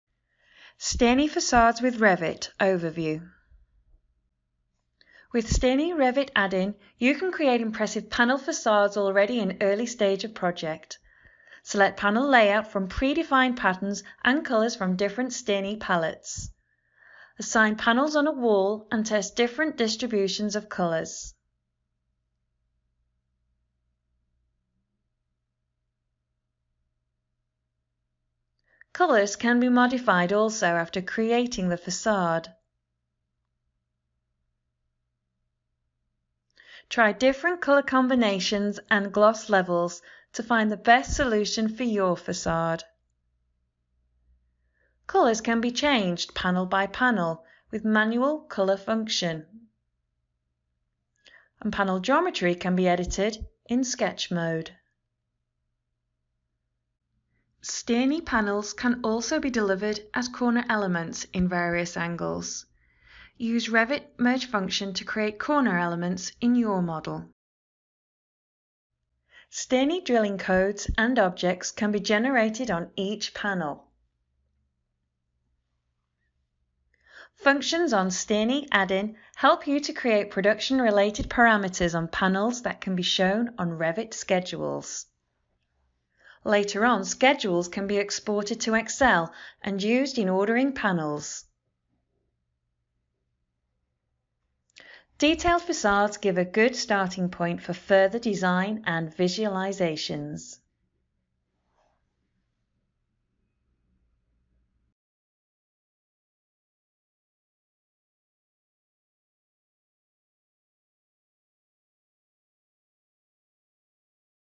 Revit Overview
Steni BIMelements for Revit. How to work 50% faster and design more advanced facades.